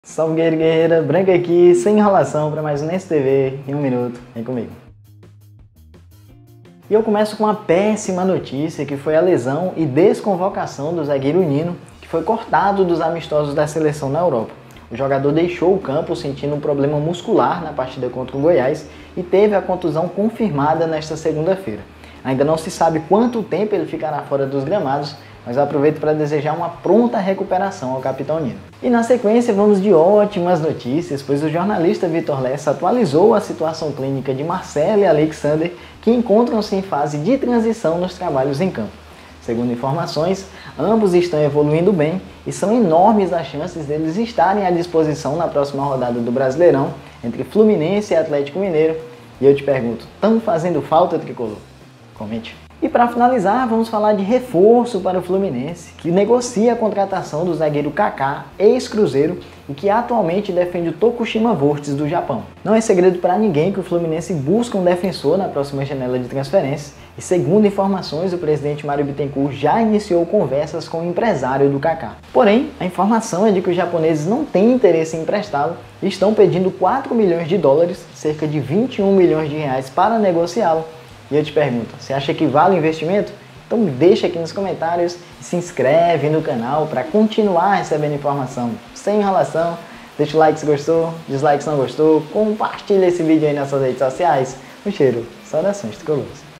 🚨URGENTE! FLU NEGOCIA CONTRATAÇÃO DE CRIA DO CRUZEIRO E MAIS NOTÍCIAS DO FLUMINENSE HOJE | FLUNEWS
Já viu essa outra música do Fluminense?

🚨 ATUALIZAÇÕES IMPORTANTES E NOTÍCIAS ANIMADORAS PARA A TORCIDA DO FLUMINENSE